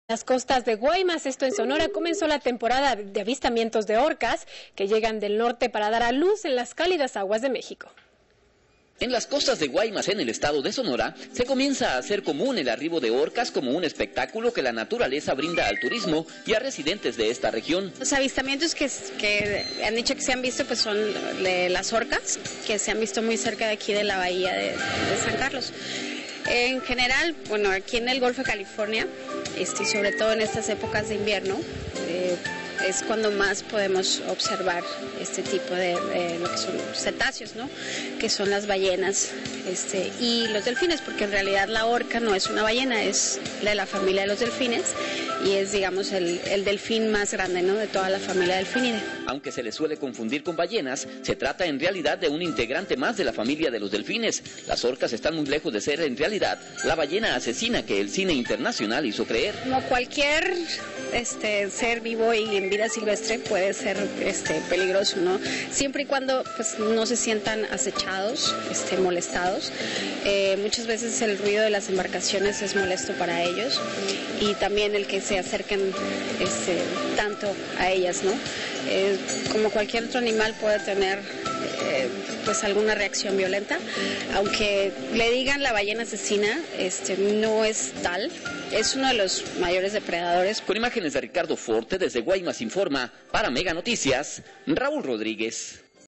Arriban orcas
En las costas de Guaymas, Sonora, comenzó la temporada de avistamiento de orcas,  que llegan del norte para dar a luz en las cálidas aguas de México...